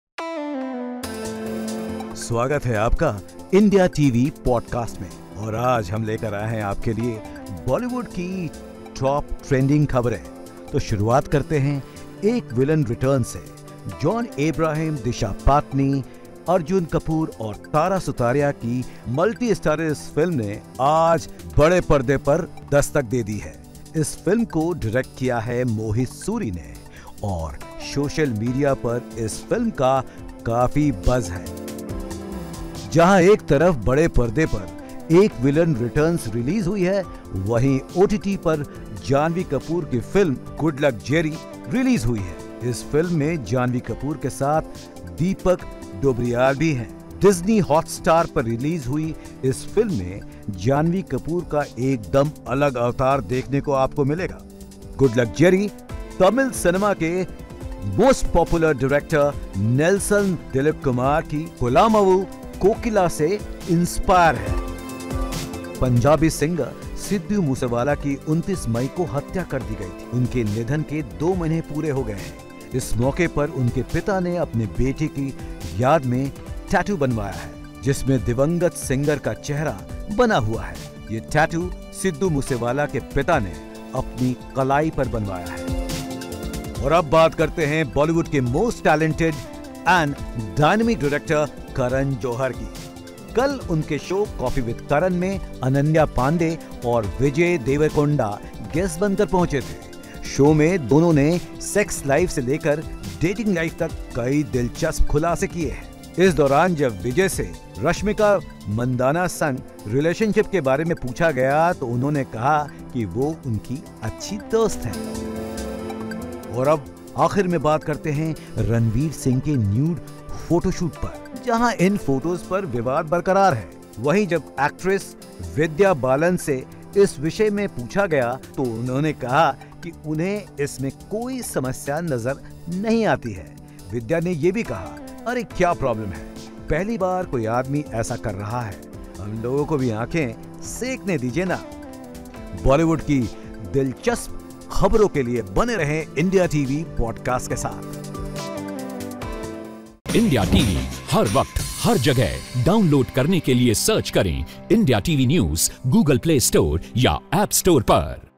IndiaTV Podcast | Top Bollywood Stories: Ek Villain Returns Hits the Big Screens | July 29, 2022
IndiaTV Presents 'IndiaTV Podcast' - a new platform where we bring to you the top stories that made headlines throughout the day. Know the top stories from Bollywood that made the buzz today.